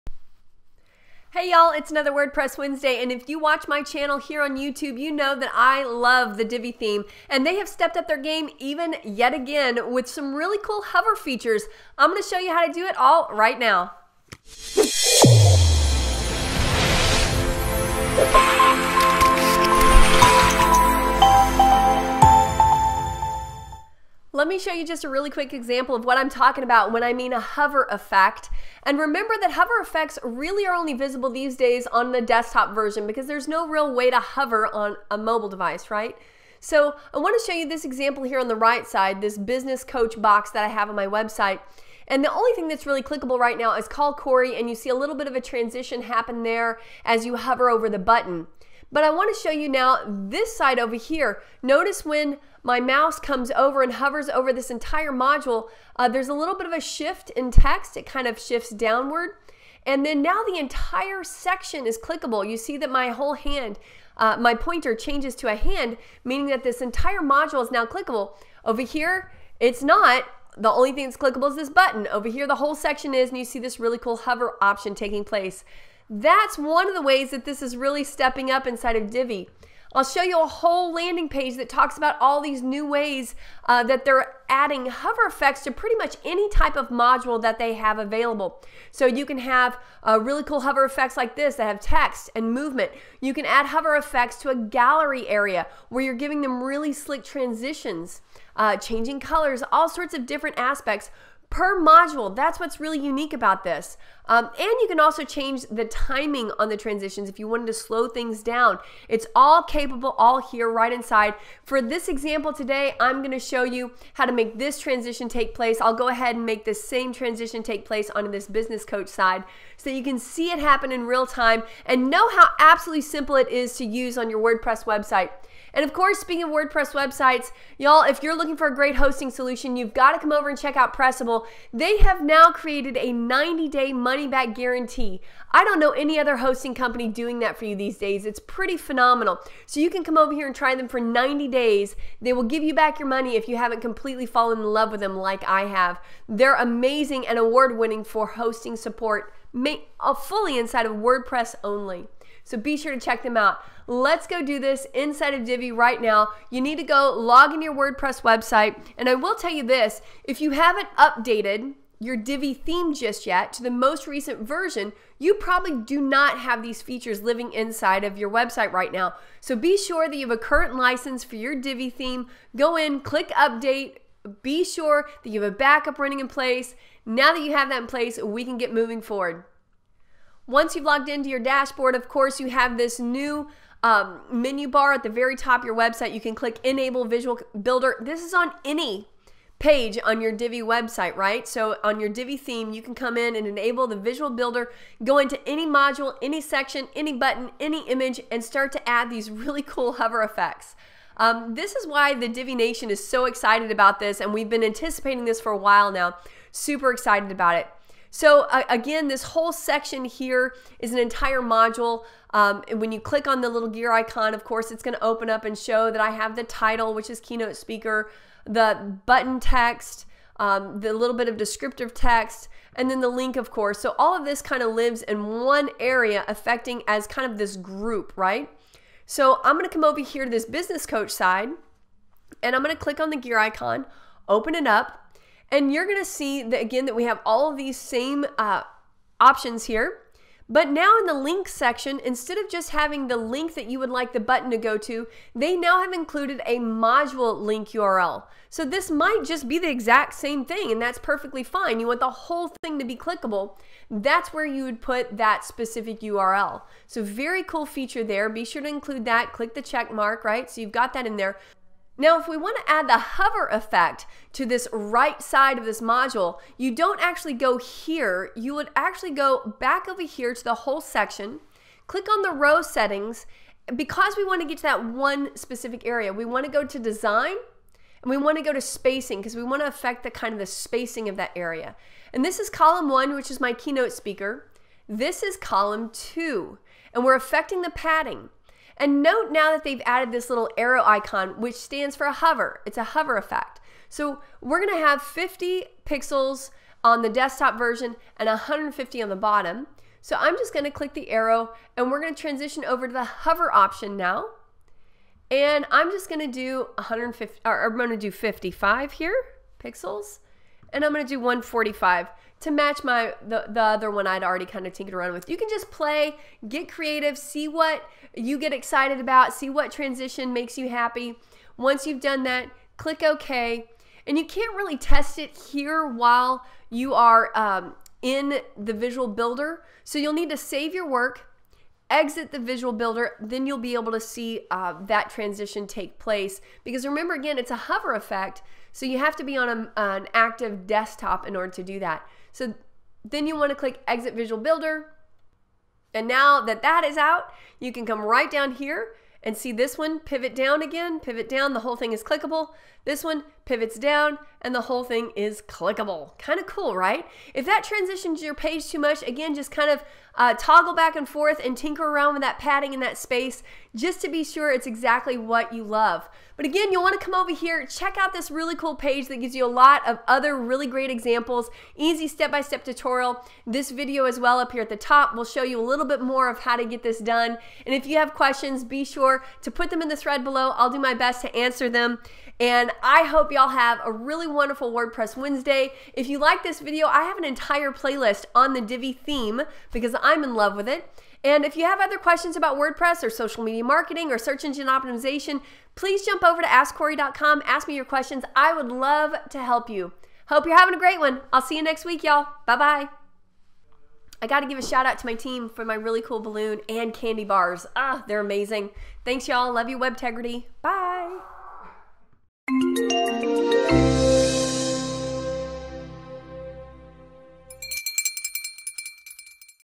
✴ NEW ✴ WordPress Divi Theme Hover Effect - Simple Steps 😄
If you're a Divi Theme user on WordPress then you need to be updating to get the newest Hover Effects in Divi! They're amazingly simple to apply and fun to use. Kori Ashton will help you apply them to your website. Here's the link to more on the Divi Hover Tutorials